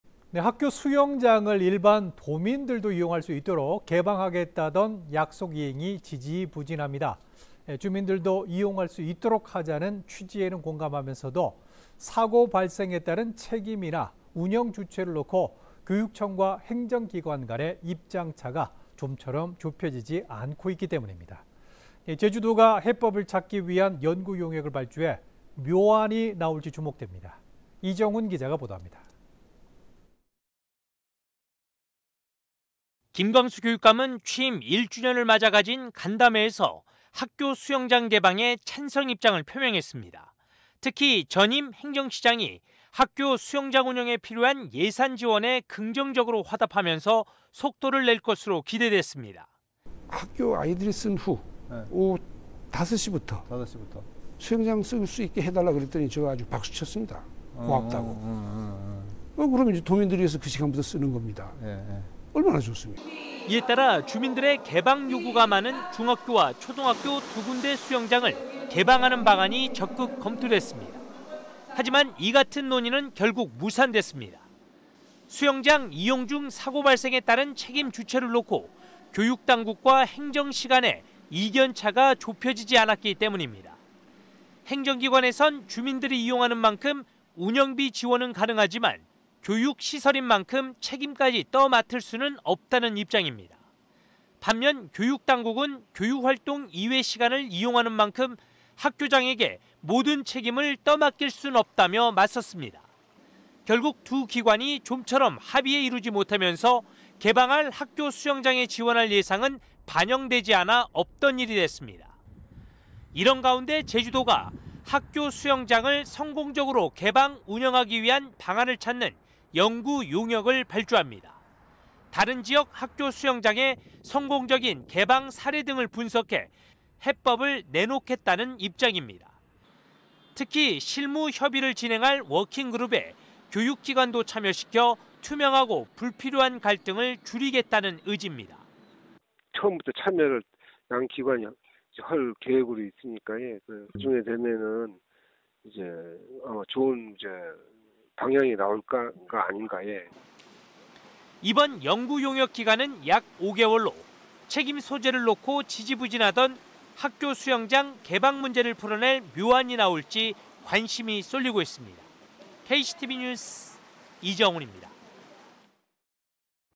학교수영장 개방 지지부진…"묘안 찾나?"
학교 수영장을 일반 도민들도 이용할 수 있도록 개방하겠다는 약속 이행이 지지부진합니다.

주민들도 이용할 있도록 하자는 취지에는 공감하면서도 사고 발생에 따른 책임이나 운영 주체를 놓고 교육청과 행정기관간에 이견이 좀처럼 좁혀지지 않고 있기 때문인데요.

제주도가 해법을 찾기 위한 연구용역을 발주해 묘안이 나올지 주목됩니다.

김광수 교육감은 취임 1주년을 맞아 가진 간담회에서 학교 수영장 개방에 찬성 입장을 표명했습니다.

특히 전임 행정시장이 학교 수영장 운영에 필요한 예산 지원에 긍정적으로 화답하면서 속도를 낼 것으로 기대됐습니다.

[김광수 / 제주도교육감 (지난해 7월)]
"제주시장께서 학교 아이들이 쓴 후 다섯시부터 수영장 쓸 수 있게 해달라고 (요청했더니) 제가 아주 박수쳤습니다. 고맙다고 도민들을 위해서 그 시간부터 쓰는 겁니다. 얼마나 좋습니까?"

이에 따라 주민들의 개방 요구가 많은 중학교와 초등학교 2군데 수영장을 개방하는 방안이 적극 검토됐습니다.

하지만 이 같은 논의는 결국 무산됐습니다.

수영장 이용 중 사고 발생에 따른 책임 주체를 놓고 교육당국과 행정시간의 이견차가 좁혀지지 않았기 때문입니다.

행정기관에선 주민들이 이용하는 만큼 운영비 지원은 가능하지만 교육시설인만큼 책임까지 떠맡을 수는 없다는 입장입니다.

반면 교육당국은 교육활동 이외 시간을 이용하는 만큼 학교장에게 모든 책임을 맡길 수는 없다며 맞섰습니다.

결국 두 기관이 좀처럼 합의에 이루지 못하면서 개방할 학교 수영장에 지원할 예산은 반영되지 않아 없던 일이 됐습니다.

이런 가운데 제주도가 학교 수영장을 성공적으로 개방 운영하는 방안을 찾기 위한 연구용역을 발주합니다.

다른 지역 학교수영장의 성공적인 개방 사례 등을 분석해 해법을 내놓겠다는 입장입니다.

특히 실무 협의를 할 워킹그룹에 교육청도 참여시켜 투명하고 불필요한 갈등을 줄이겠다는 의집니다.

[제주도청 관계자]
"처음부터 참여를 양 기관이 할 계획으로 있으니까 나중에 되면 아마 (학교 수영장 개방에) 좋은 방향이 나오지 않을까"

이번 연구용역 기간은 약 5개월로 책임소재를 놓고 지지부진하던 학교 수영장 개방 문제를 풀어낼 묘안이 나올지 관심이 쏠리고 있습니다.